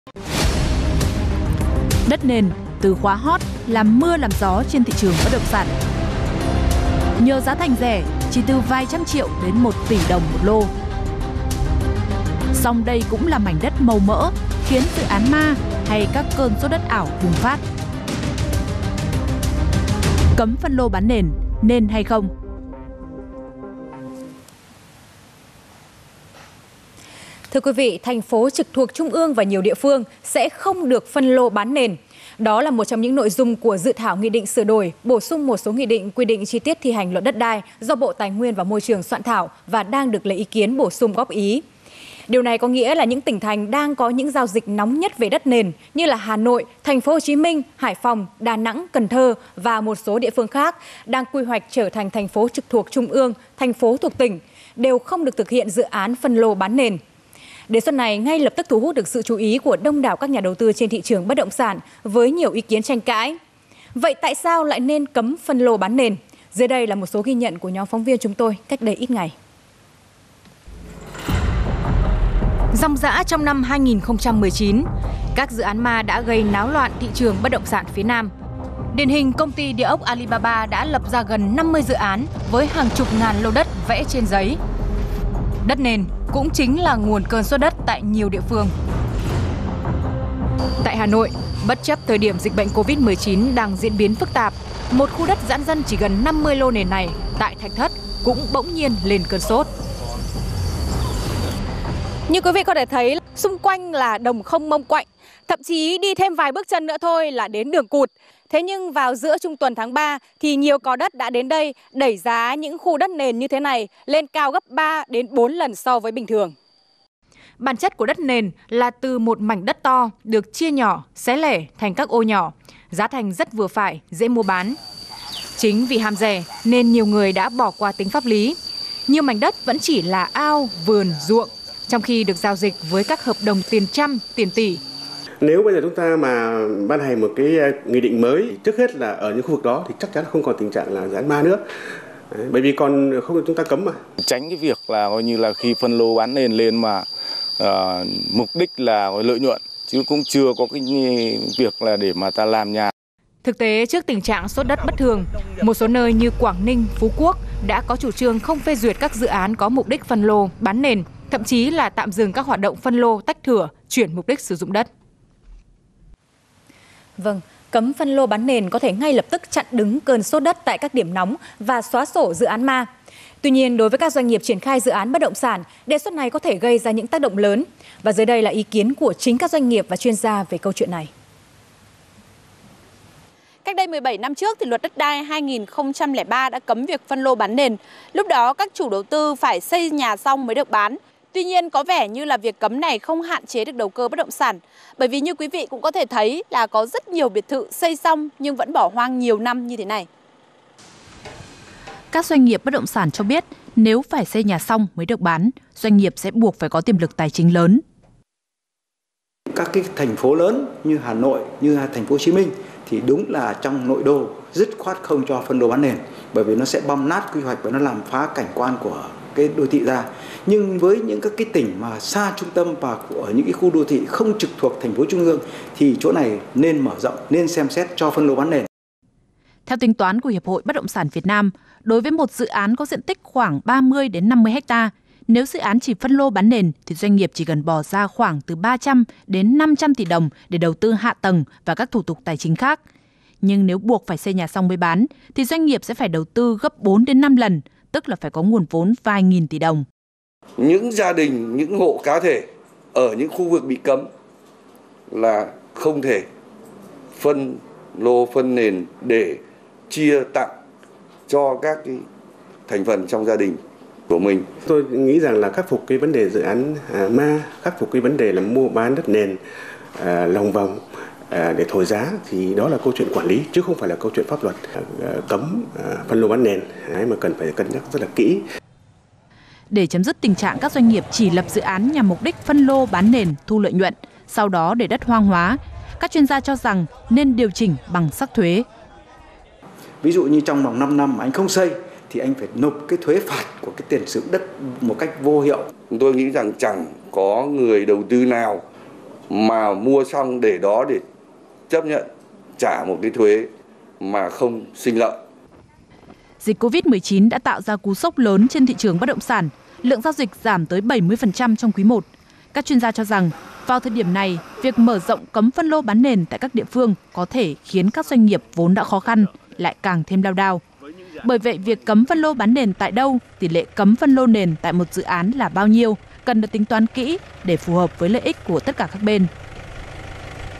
Cấm phân lô bán nền liệucó “xóa sổ” được dự án ma? | VTV24
"ĐẤT NỀN” – từ khóa hot, làm mưa làm gió trên thị trường BĐS.
Nhờ giá thành rẻ, chỉ từ vài trăm triệu đến 1 tỷ đồng/1 lô.
Song, đây cũng là “mảnh đất màu mỡ” khiến dự án ma, hay các cơn sốt ảo bùng phát.